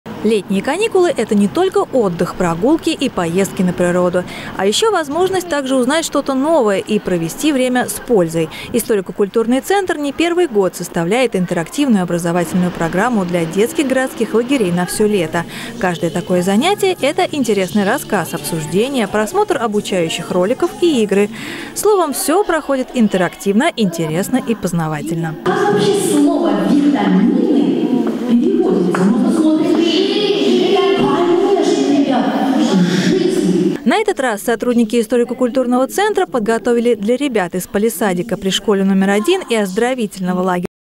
БЫТЬ ЗДОРОВЫМ
Что такое витамины, и какие они бывают? Как часто нужно мыть руки? Можно ли укрепить иммунитет и как это сделать? Об этом и многом другом ребята из городских летних лагерей узнали на интерактивном занятии «Быть здоровым – здорово» в Историко-культурном центре.